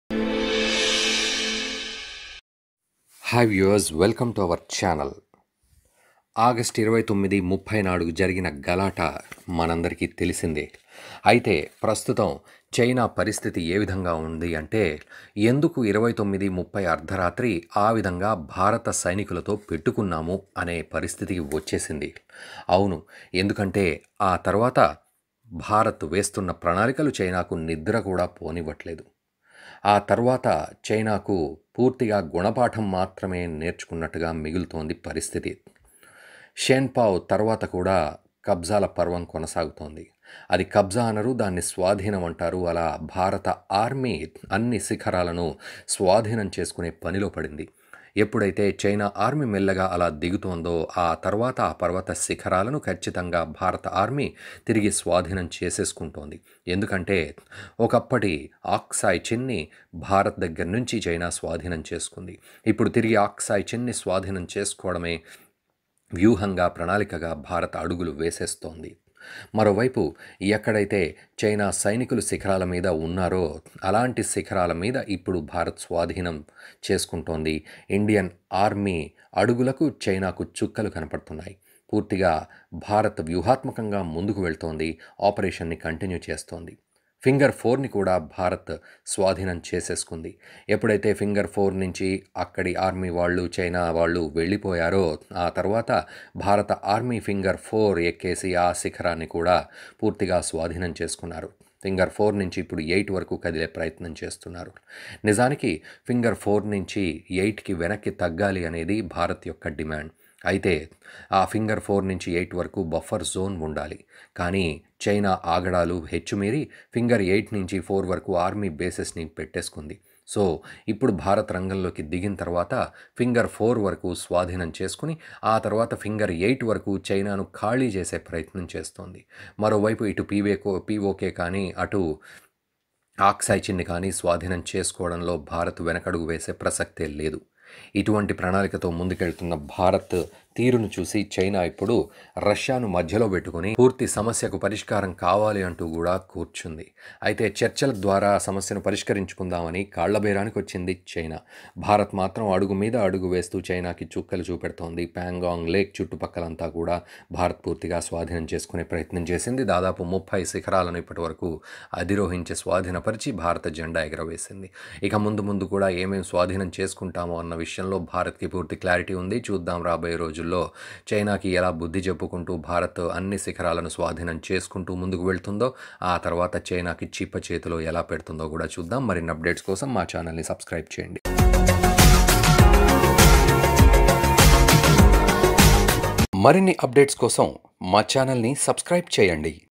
రాత్రి 2 గంటలకు ప్రారంభించిన ఘర్షణ..సరిహద్దుల్లో చైనా బలగాల వెన్నులో మొదలైన వణుకు || India Vs China
Watch: రాత్రి 2 గంటలకు ప్రారంభించిన ఘర్షణ..సరిహద్దుల్లో చైనా బలగాల వెన్నులో మొదలైన వణుకు || India Vs China